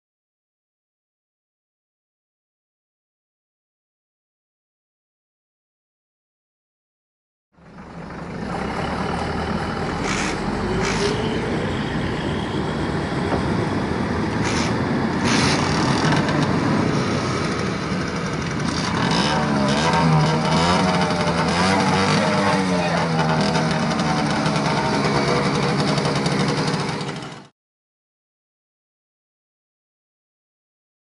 Abfahrt eines Zuges in Väderum
Die schmalspurbahn Hultsfred - Västervik führt in den Sommermonaten regelmässige Fahrten mit den historischen Triebwagen der Bauart YP durch. In Väderum sind wir dem Museumszug begegnet und haben ihn bei der Abfahrt beobachtet.